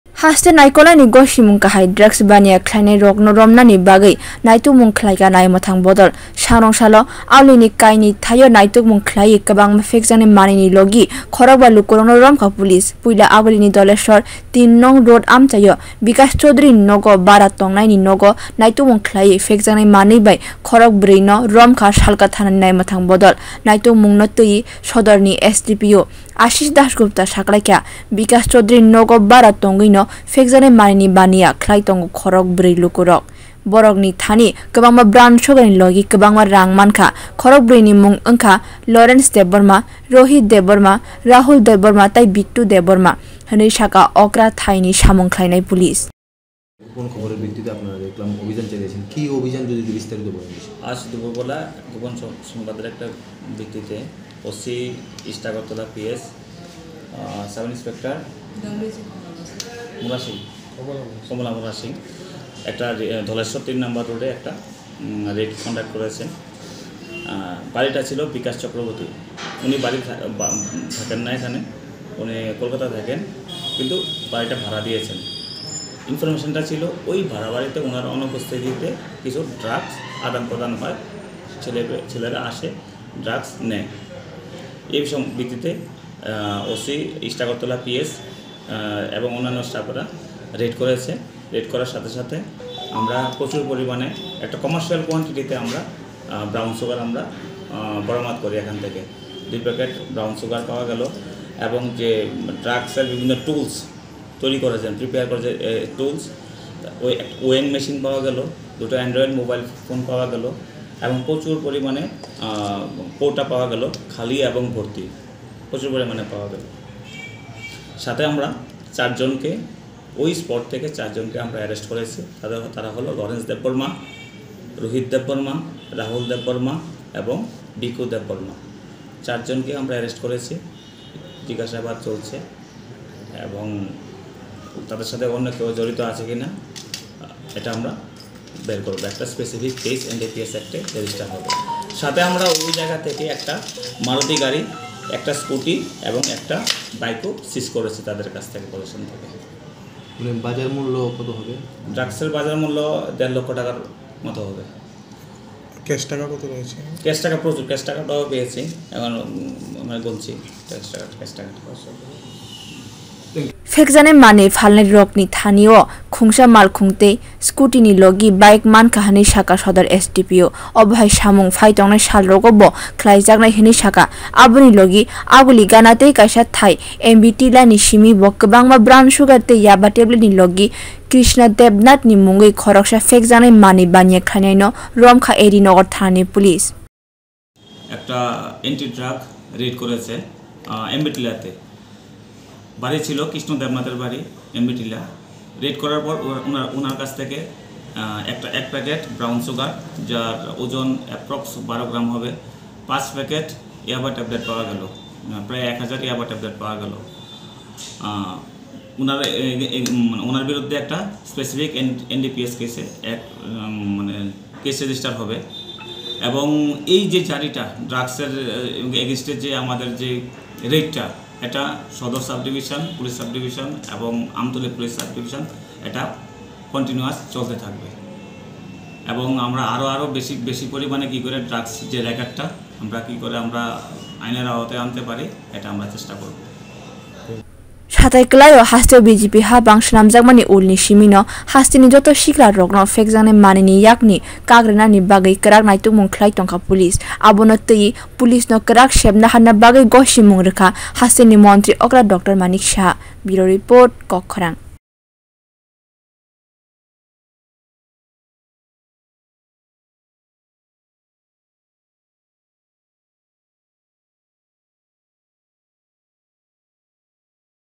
Kainwi thaio raid khwlaikha police.
Aguli ni kainwi thaio no naitokmwng khwlawi sangrong salo khorok ba drug’s phalnai no romkha police, borok ni thani kwbangma phekjak nai manwi ni logi rang mankha police.